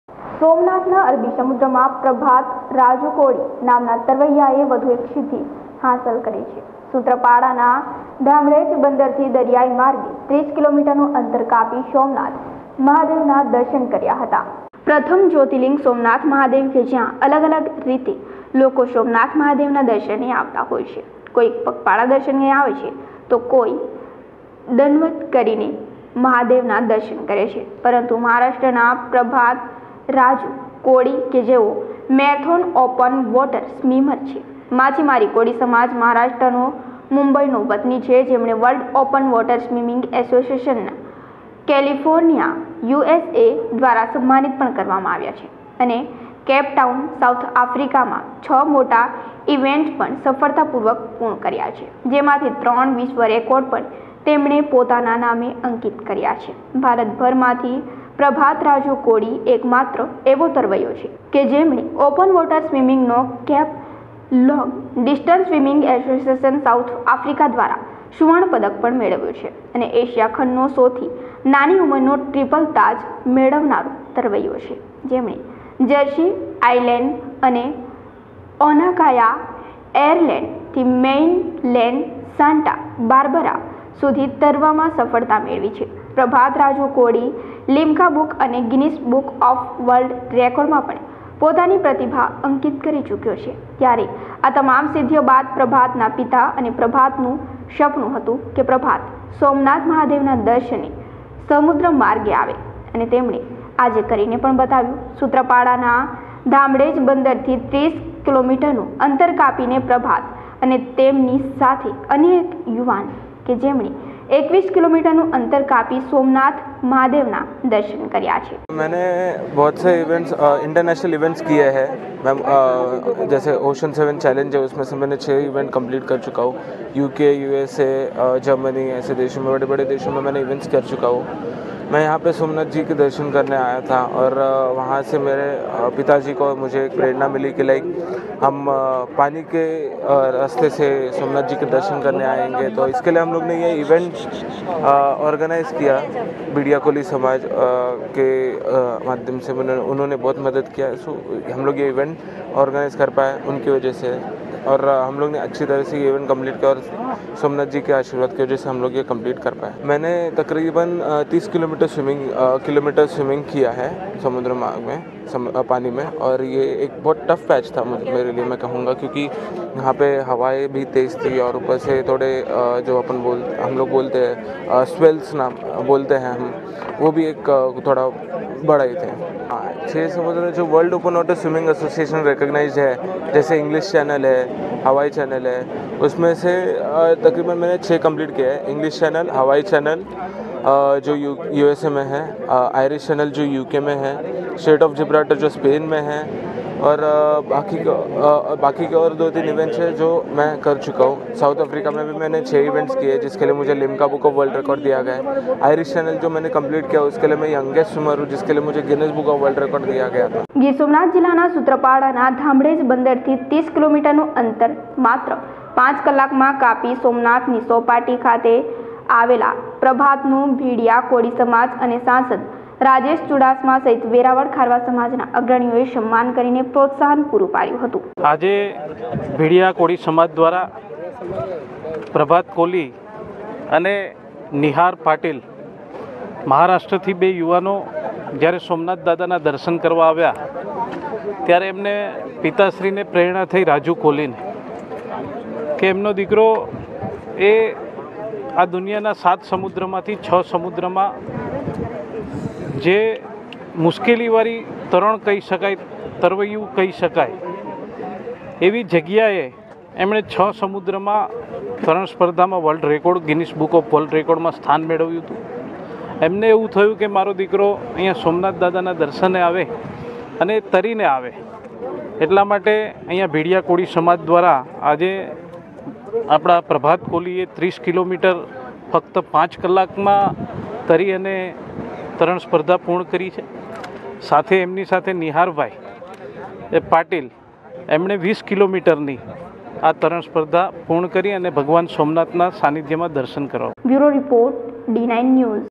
ગીર સોમનાથ : અરબી સમુદ્રમાં પ્રભાત રાજુ કોળી નામના તારવ્યા એ વધુ સીધી કરી હાસલ
Title:  ગીર સોમનાથ : અરબી સમુદ્રમાં પ્રભાત રાજુ કોળી નામના તારવ્યા એ વધુ સીધી કરી હાસલ
Name of the Reporter :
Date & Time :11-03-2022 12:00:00

Descriptions